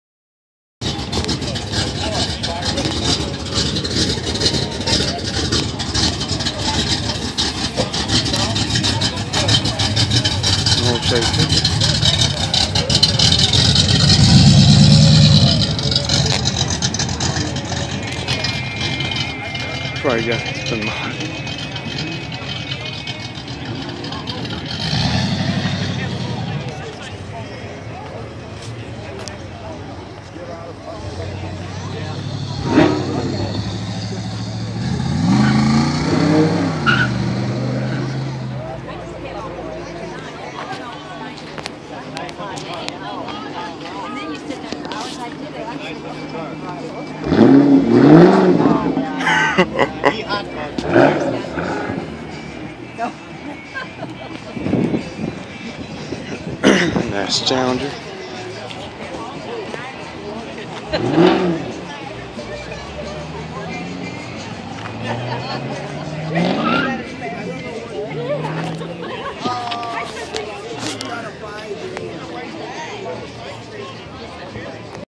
Loud Cammed Chevy 350 Silverado leaving the Culvers car show
Here's a video that I made last year of a very loud Chevy pickup with a hot cam and no exhaust it sounded awesome man.